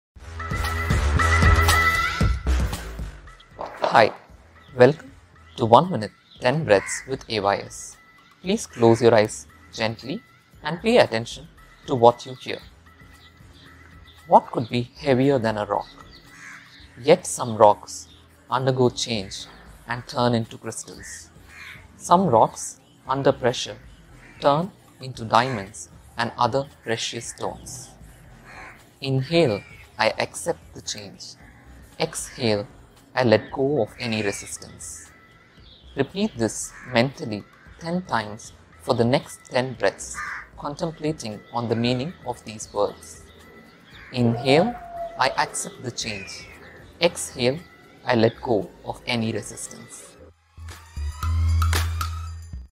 One Minute 10 Breaths with AYS
Resistance.

Listen to the instructions in the video.
Close your eyes and contemplate on the meaning of words during inhale and exhale.

Do this for 10 Breaths.
Your daily dose of meditation is now complete.

Atma Yoga Shala
Live. Love. Learn